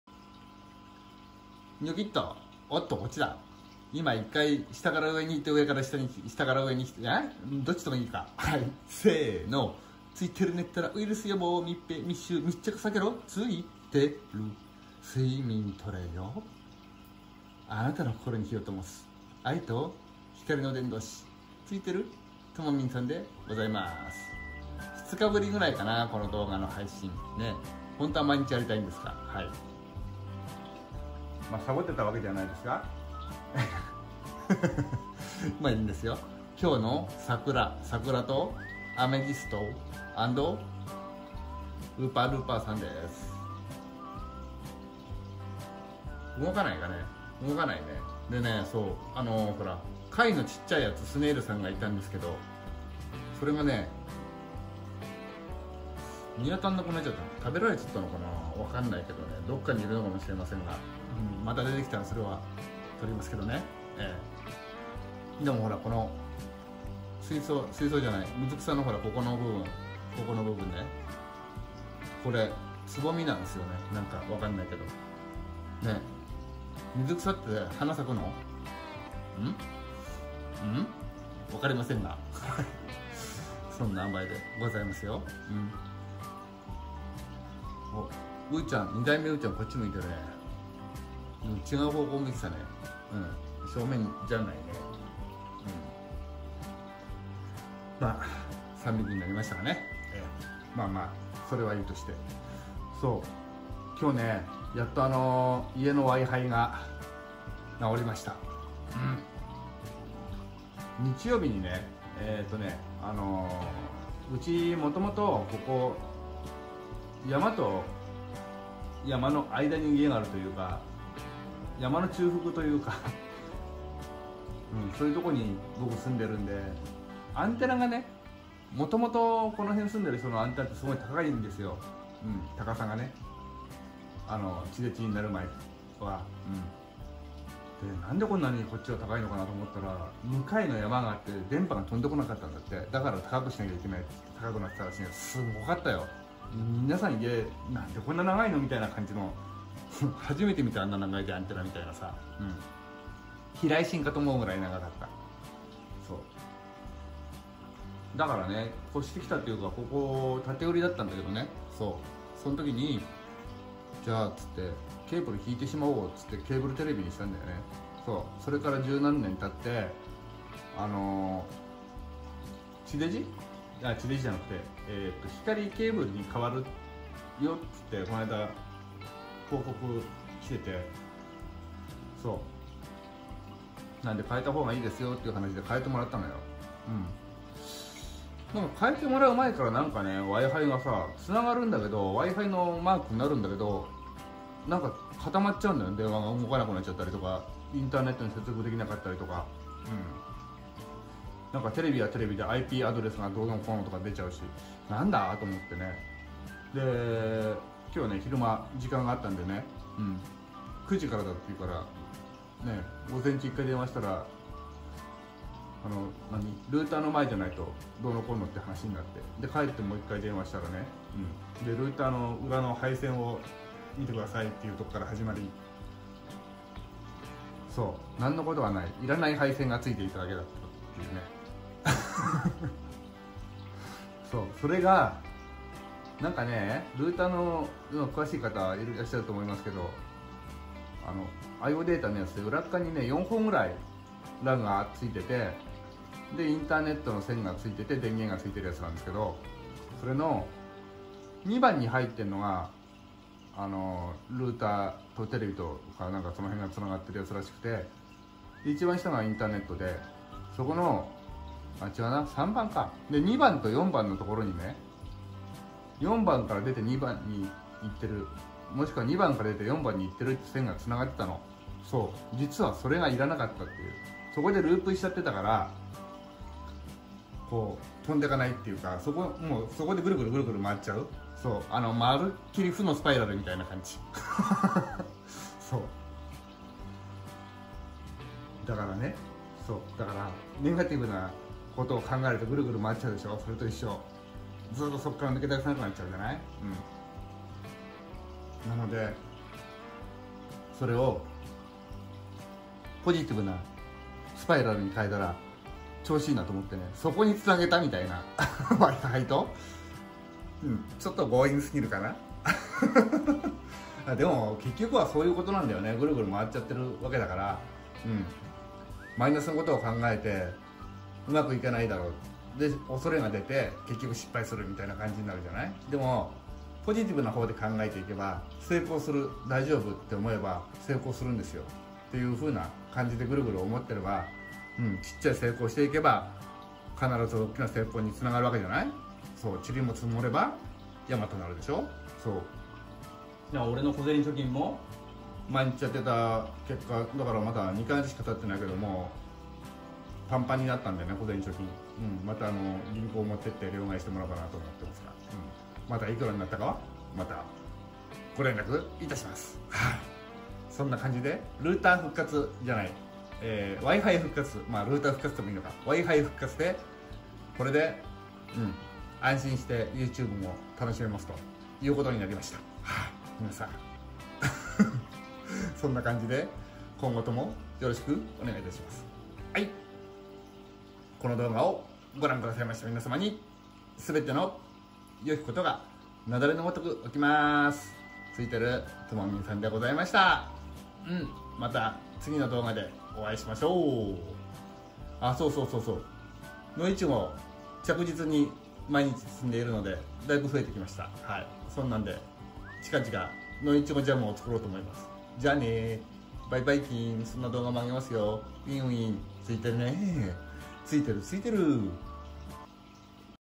457、WiFi復活‼️
不要な配線のお陰様で、全く使えないWiFiでした❗️やっと復活しました😊👍
これで、YouTubeもzoomも怖くないです😁✨